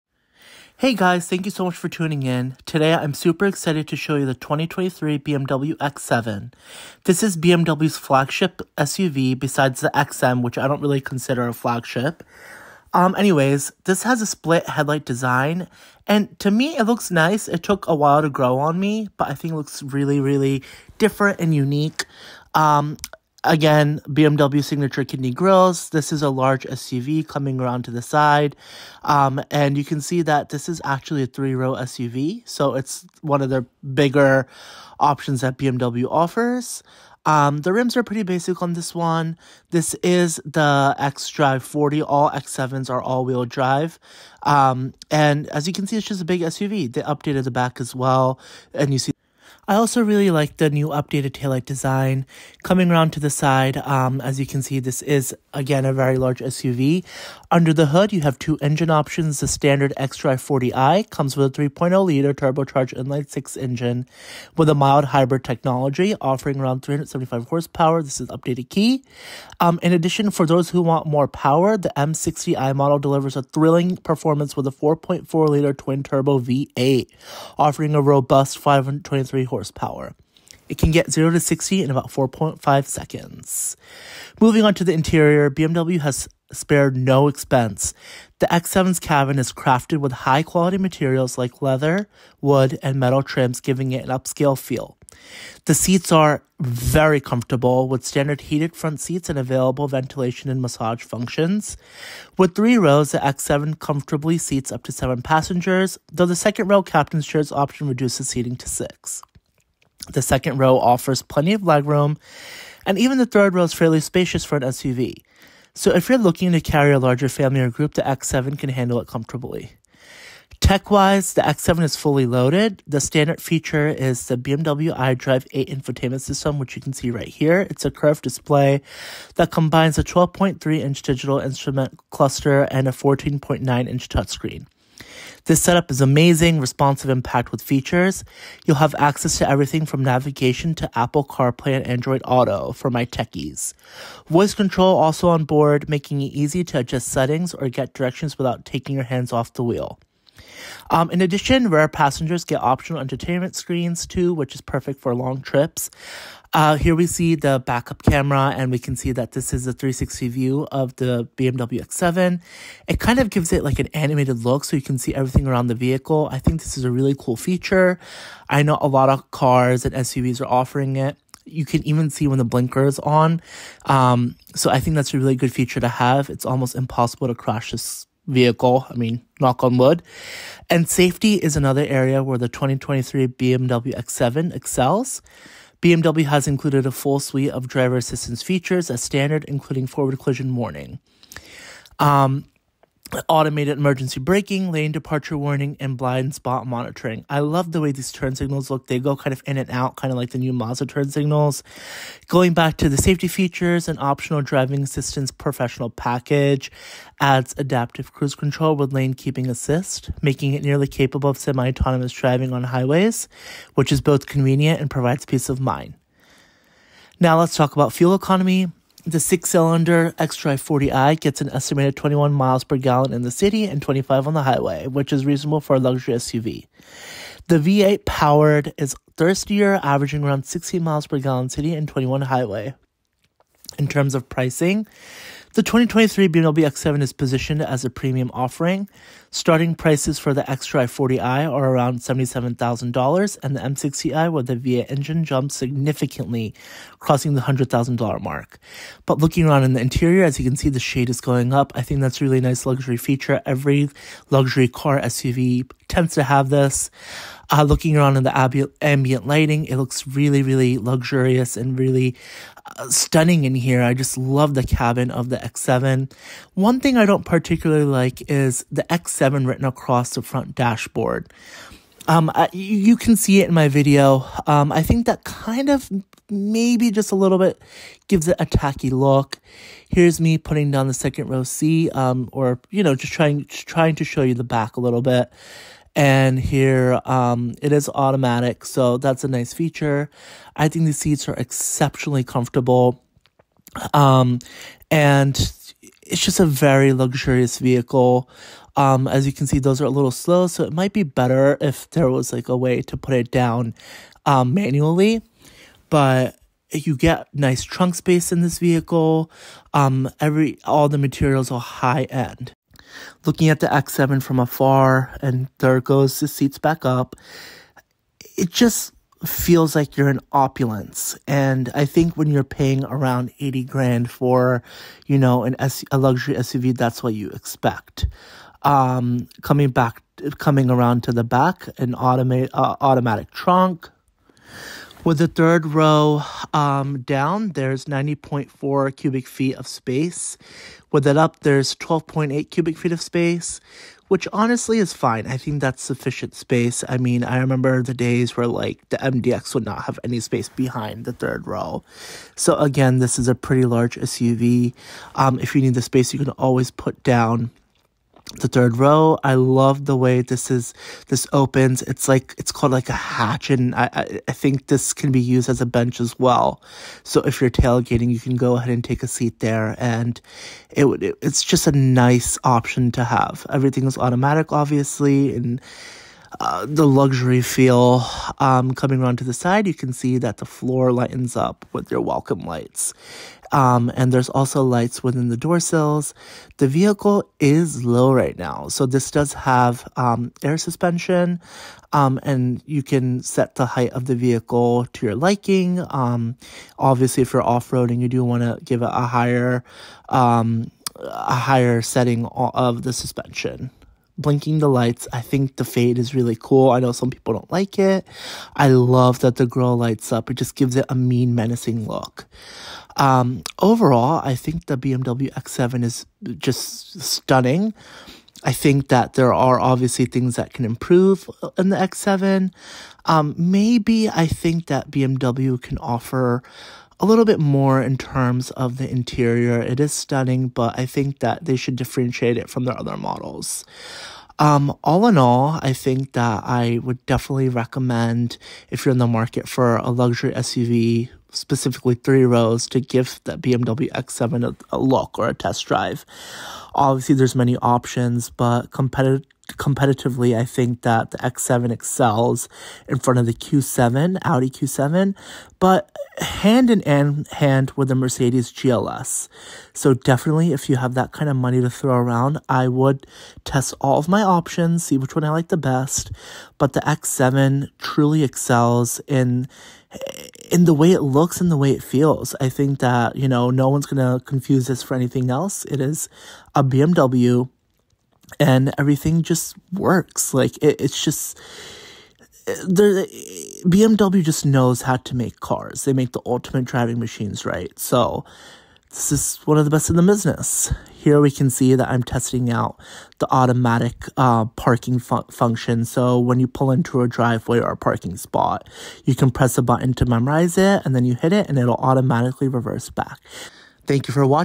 2024 BMW X7 Review: The Ultimate Luxury SUV?!
🚗 Experience the 2023 BMW X7 Like Never Before! 🚗

In this in-depth review, we dive into everything the 2023 BMW X7 has to offer, from its bold design to powerful performance. Find out why this luxury SUV stands out in its class with advanced tech, a spacious interior, and impressive driving dynamics!

Watch until the end for the full experience!